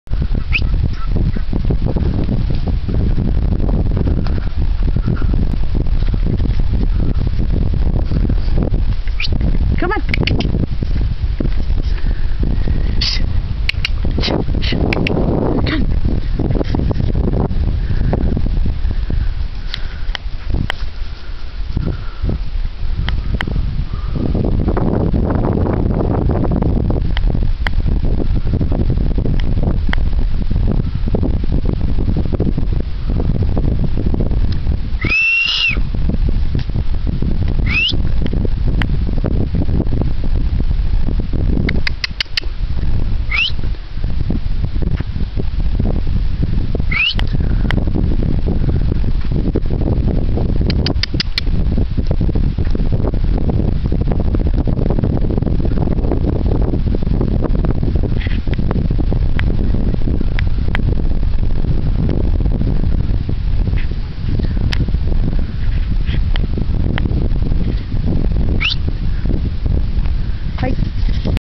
SALEM AND SAM STRUTTING BABY ARABIAN STYLE
Two 2009 Black Straight Egyptian yearlings showing off for all the world to see.
These are an example of our home-grown, quality Arabians. Sam has three socks, Salem has two hind socks.